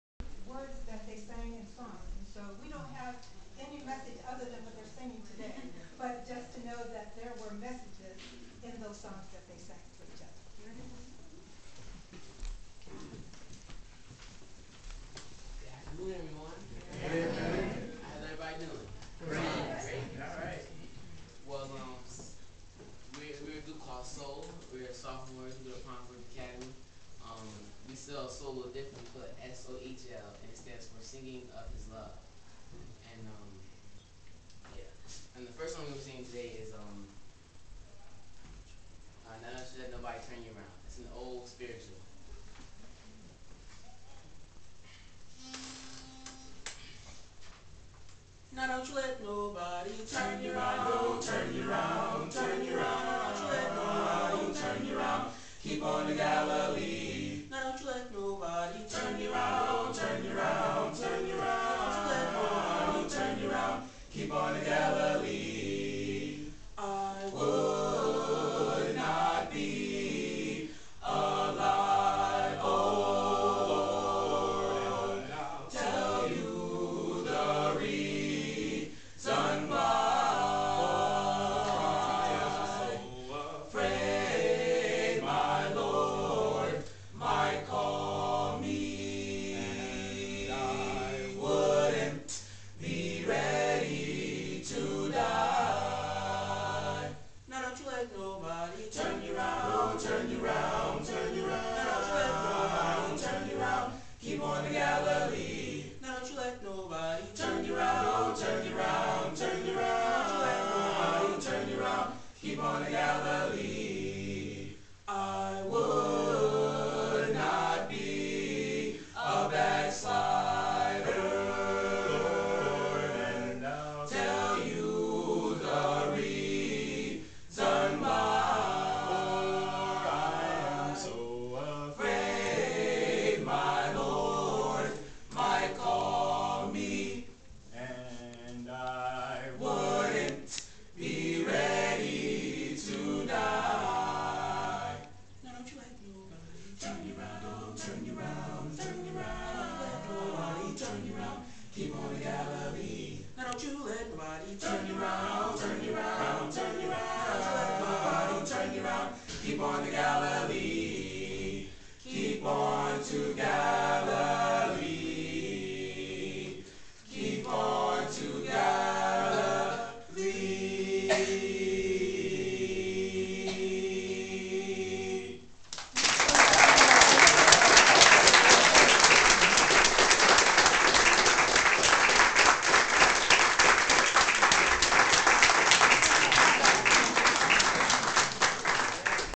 Pine Forge Academy Historical Society, Male Vocal
I took this video during the Pine Forge Historical Society's  (Pine Forge, Pa.) Second Annual Berks County Underground Railroad Tour on April 26, 2009.  The tour was organized in conjunction with the Central Pennsylvania African American Museum (CPAAM), located in Reading, Pa.  Here in this video is featured the Pine Forge Academy's male vocal group singing Black spirituals of the underground era.